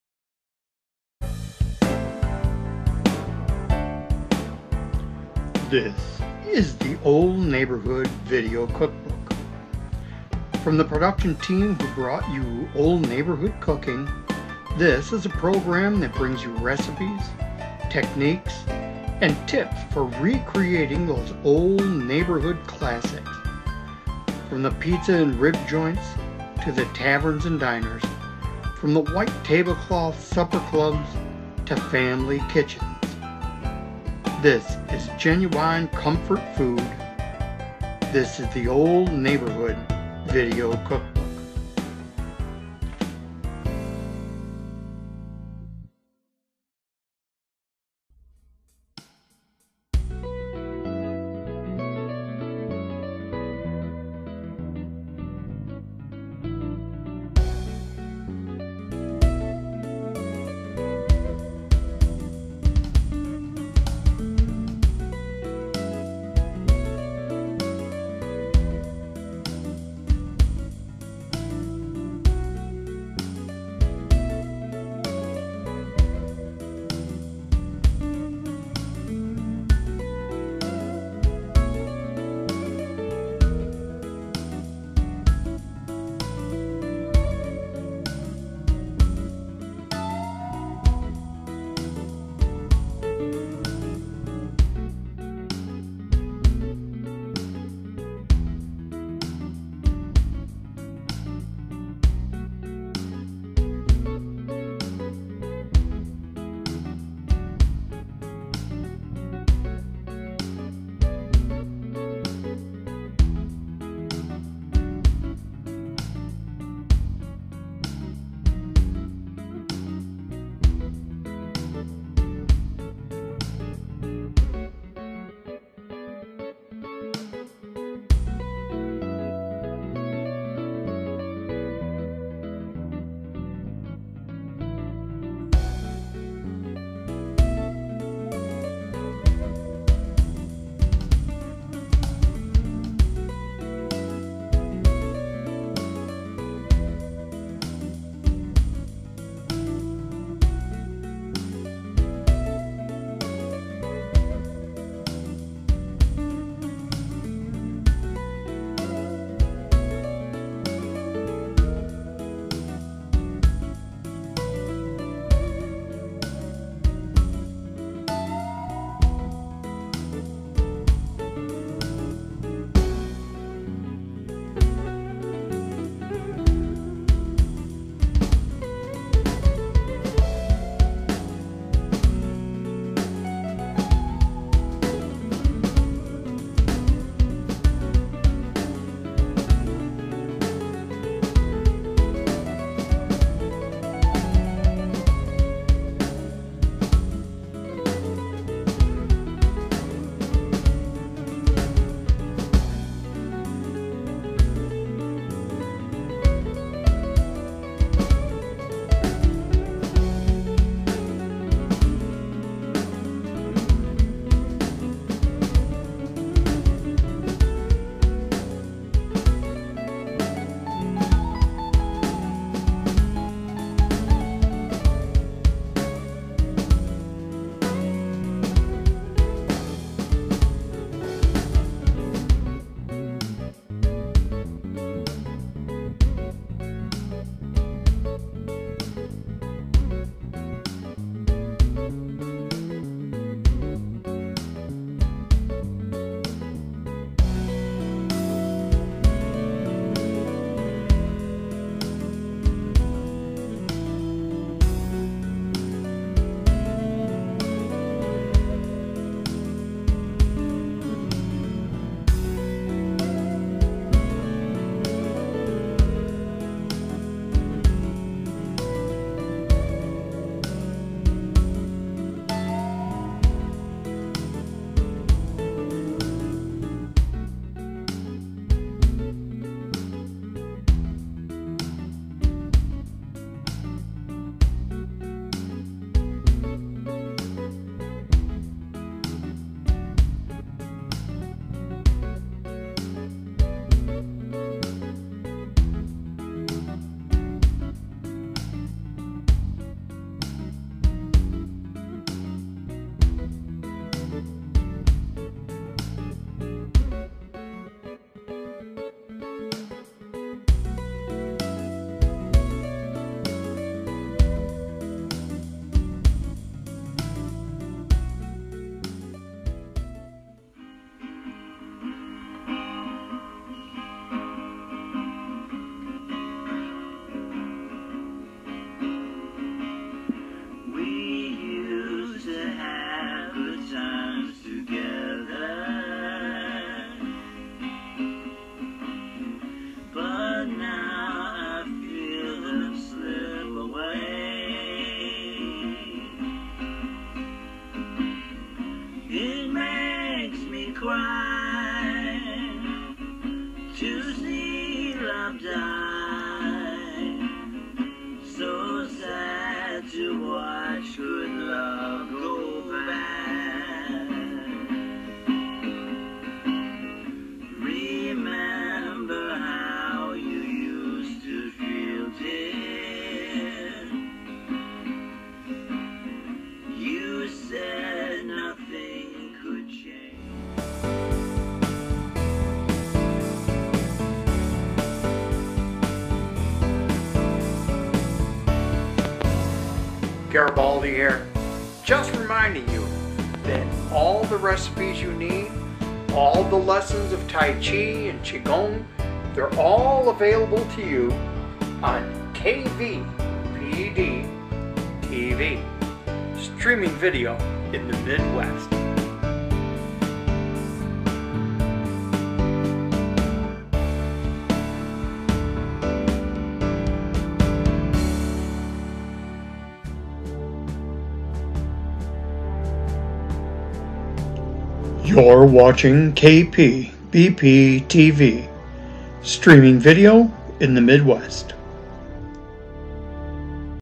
E 4 Naples Bakery 1968
2 breads from the Italian bakery on the eastside of Detroit, circa 1968 - Old World Italian bread and "Walkin' Around" Focaccia stuffed with spicy pepperoni